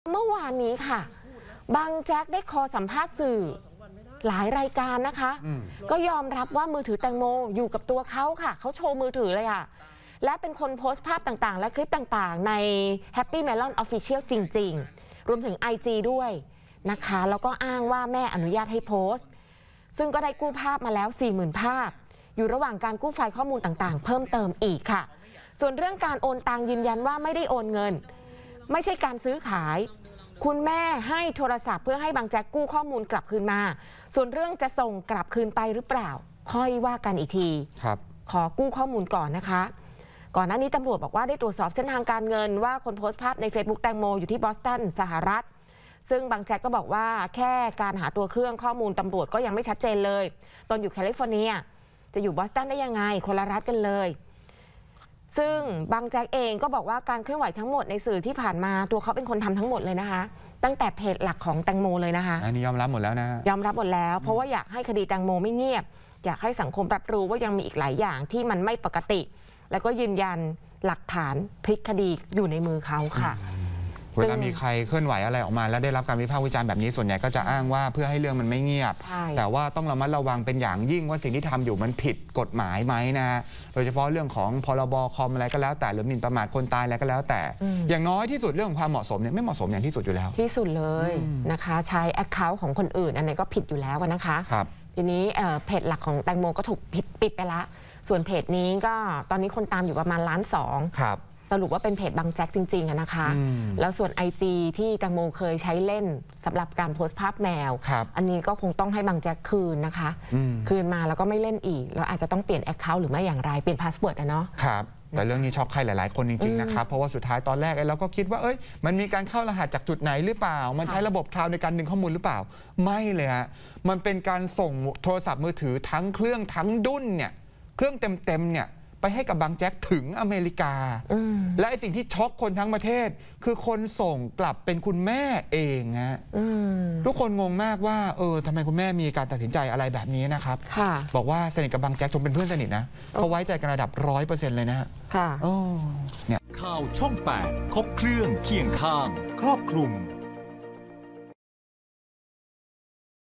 " บังแจ็ค " เปิดหน้ายอมรับ โพสต์โซเชียล " แตงโม " | ข่าวช่อง 8 | 26 พ.ค. 65
เมื่อวานนี้ บังแจ็ค วิดีโอคอล ให้สัมภาษณ์สื่อหลาย รายการ ยอมรับแล้วว่า โทรศัพท์แตงโม อยู่กับตัวเองจริง และ เป็นคนโพสต์ภาพและคลิปต่าง ๆ ในเฟซบุ๊ก Happy Melon official และ อินสตาแกรม โดยอ้างว่าแม่อนุญาตให้โพสต์ ซึ่งได้กู้ภาพกว่า 4 หมื่นภาพ และ อยู่ระหว่างกู้ไฟล์ข้อมูลต่าง ๆ ด้วย